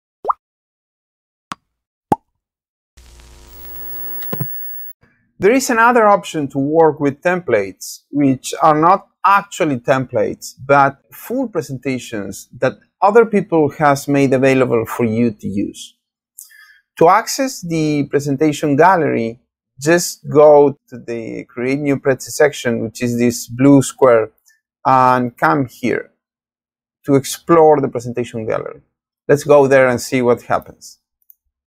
Prezi Tutorial: Prezi Gallery and Free Templates
The Prezi Gallery is an excellent alternative to the Prezi Templates.

It is a complete library where you can access already made presentations to copy and reuse in your project.

Free Prezi Tutorial!!
Learn more about Prezi Licenses, Masterclass Sessions, Free Templates, Ask the Teacher a Question and Everything Prezi with our Free Prezi Tutorial